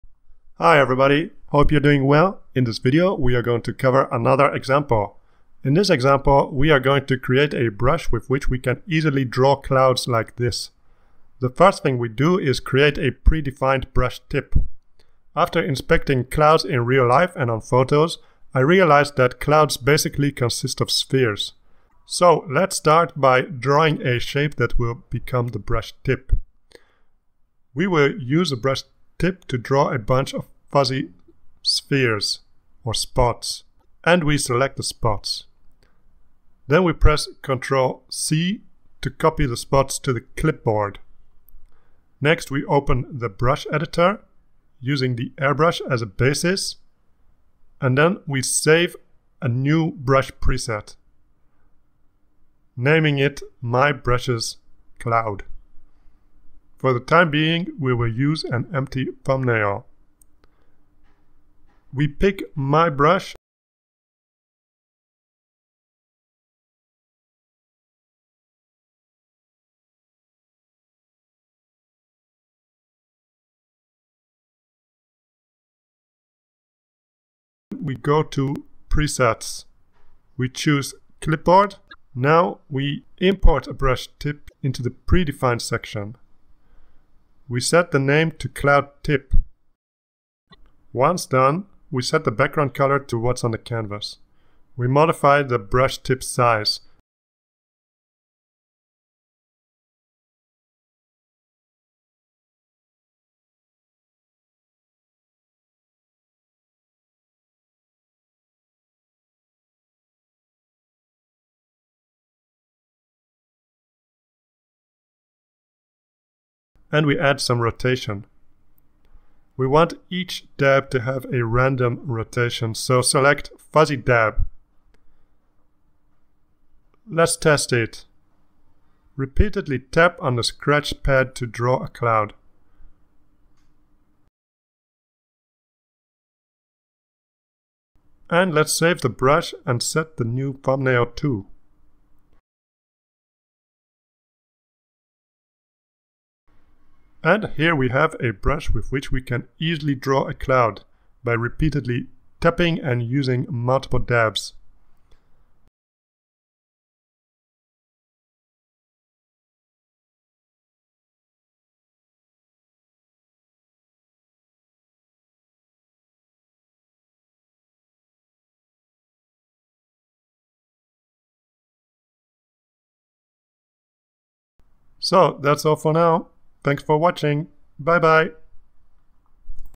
Krita Custom Brushes - Cloud Brush
In this video we will be making a custom brush with which we can easily and quickly draw clouds in Krita.

Or Udemy. Use this link to get the full course at a discount!